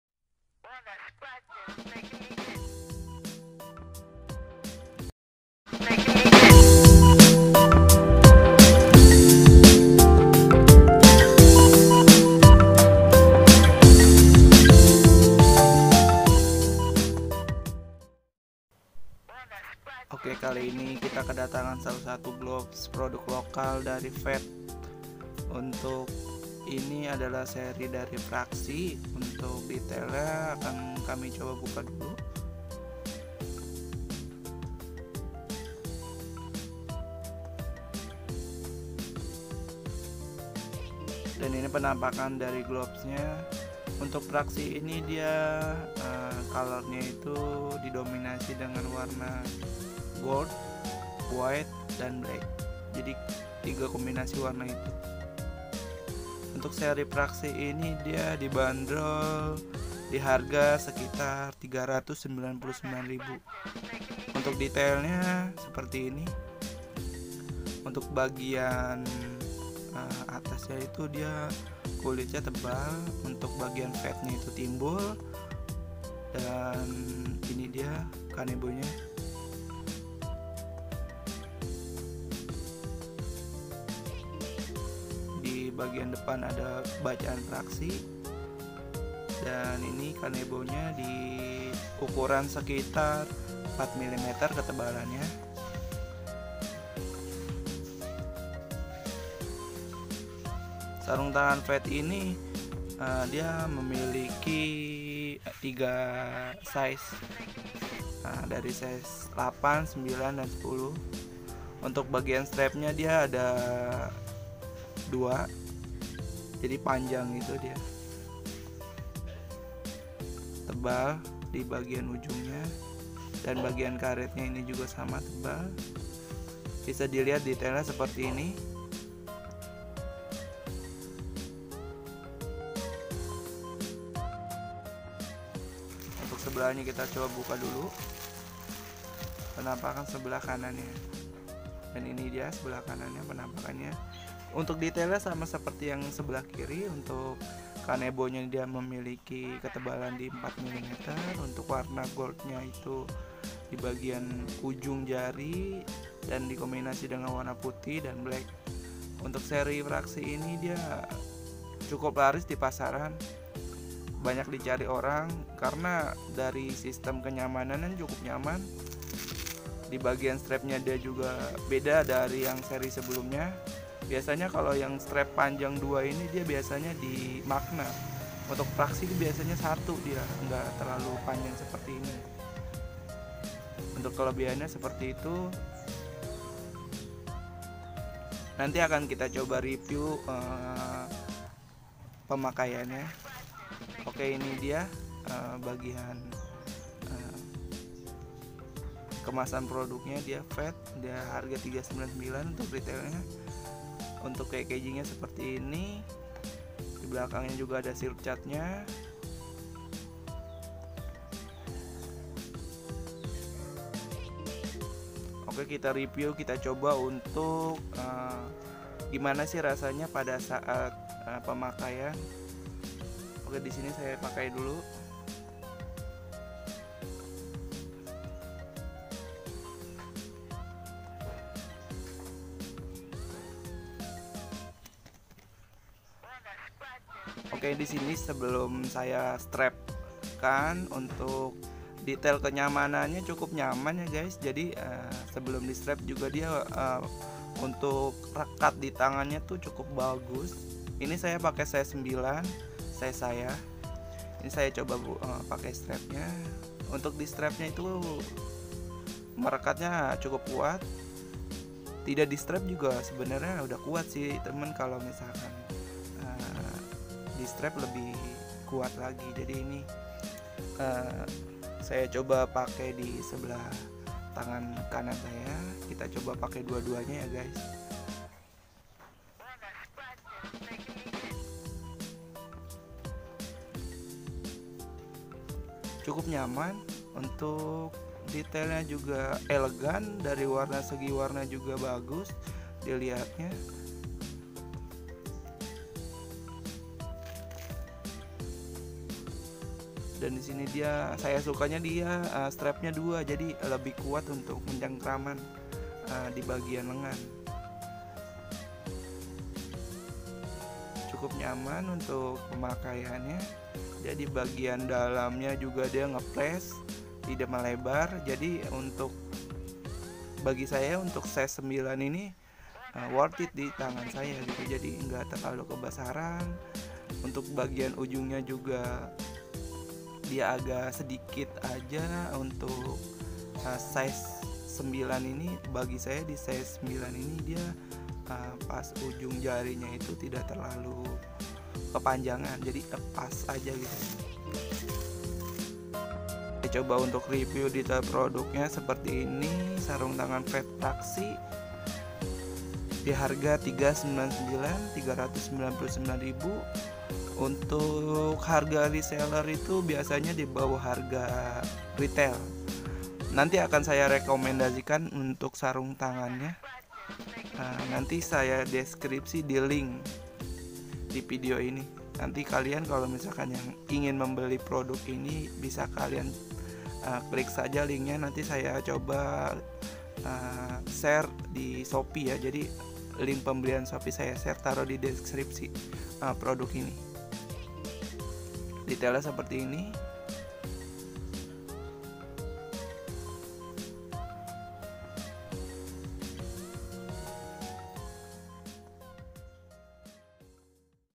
FAT PRAXI GOLD | SARUNG TANGAN KIPER TERBARU | PRODUK LOKAL DENGAN HARGA YANG AMAN DI KANTONG
FAT PRAXI GOLD SARUNG TANGAN TERBARU PRODUK LOKAL TERNYAMAN DENGAN HARGA YANG AMAN DI KANTONG

Unboxing & Review

Terima kasih ya sudah menonton video ini sampai